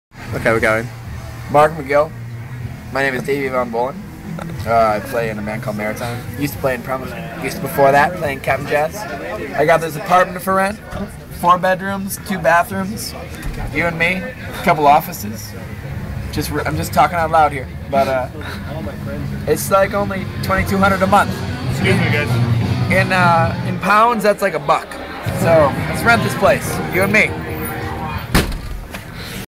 To Mark Magill from Davey Von Bohlen
A video message for Mark Magill from Davey Von Bohlen outside The Subteranean, Chicago.